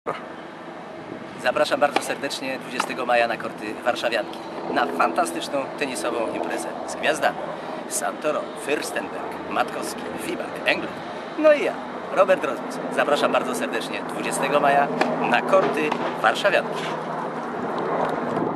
Robert Rozmus zaprasza na Siemens AGD Tennis Cup 2011
Robert Rozmus zaprasza na tenisowy turniej gwiazd Siemens AGD Tennis Cup 2011. 20 maja 2011 na korcie głównym Warszawianki  pojawią się światowe gwiazdy i legendy tenisa: Mariusz Fyrstenberg, Marcin Matkowski, Fabrice Santoro oraz Wojciech Fibak. Zagrają też najmłodsi zawodnicy Siemens AGD Tennis Team Polska: Gresk, Gawron, Walków, Gajewski. Zapraszamy! WSTĘP WOLNY